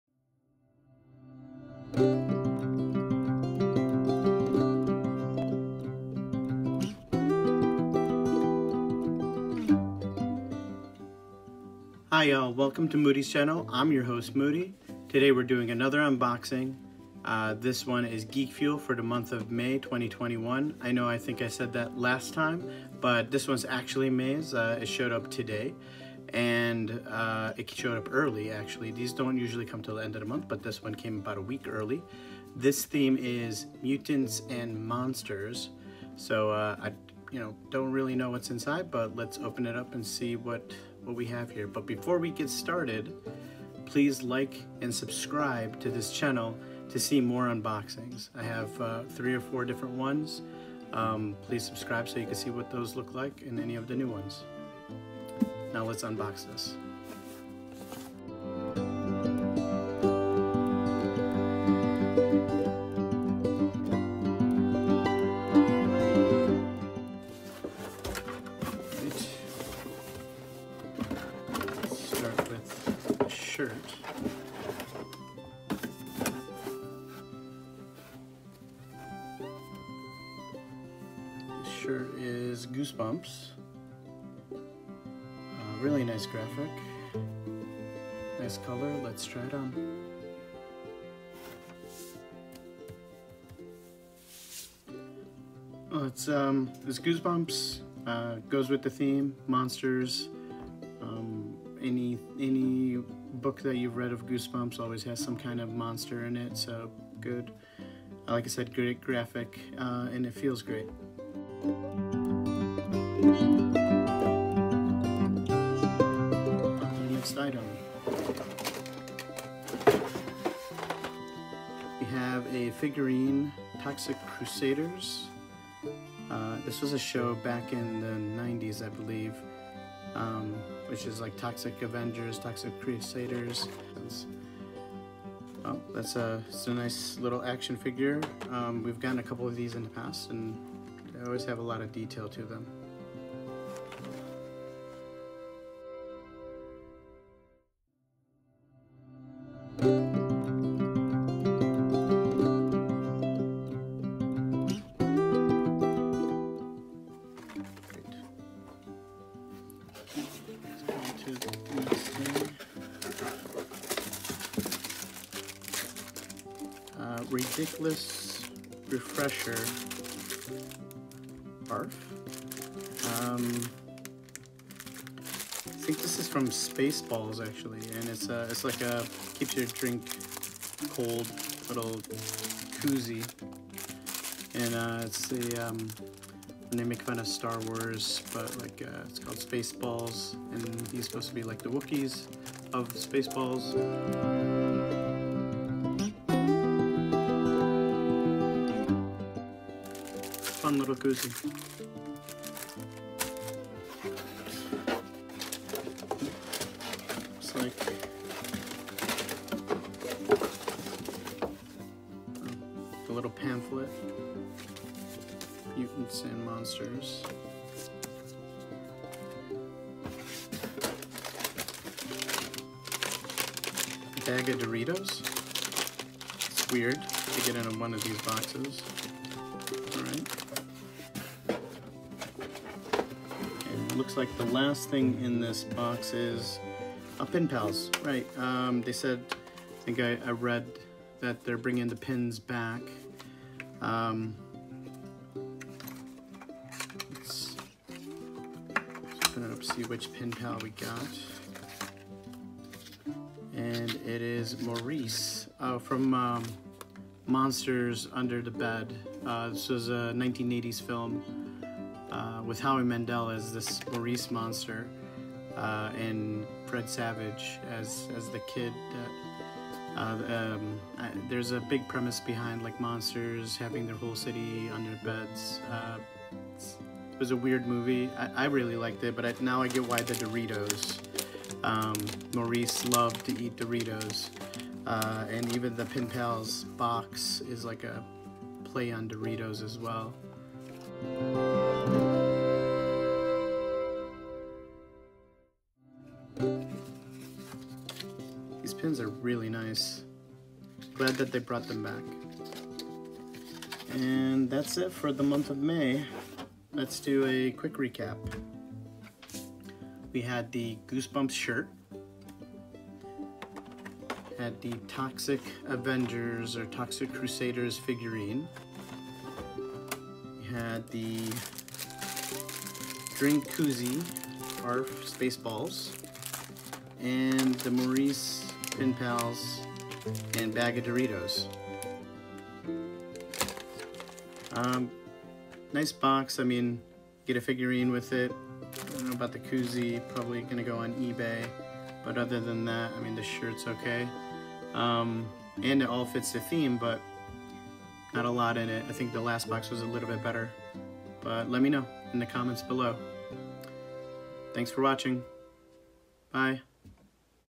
Geekfuel Unboxing May 2021 Monsters and Mutants
This box came a week early comes see what monsters and mutants we got in this box and if you like it you still have time to order and get these items!